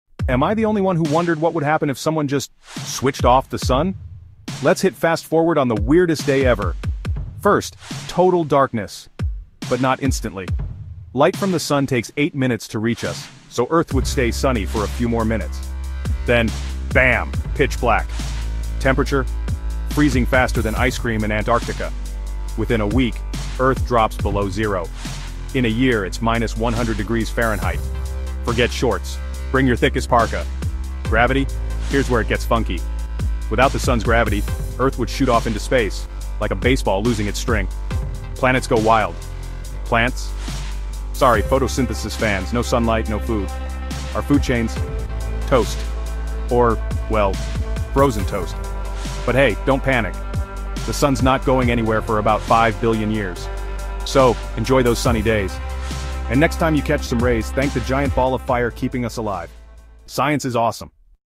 🌑 What If the Sun Suddenly Turned Off? The Chilling Consequences for Earth 🌍❄️
What would really happen if the Sun suddenly switched off? In this video, we explore the immediate effects, starting with total darkness engulfing Earth just 8 minutes after the Sun disappears. Discover how temperatures would plummet rapidly, creating extreme cold conditions that would be unfit for most life forms.

We also dive into the catastrophic impact on gravity and food chains, explaining how the absence of sunlight would disrupt ecosystems and spell disaster for all living beings. If you're fascinated by space, science, and the survival of our planet, this video breaks down the chilling consequences in detail.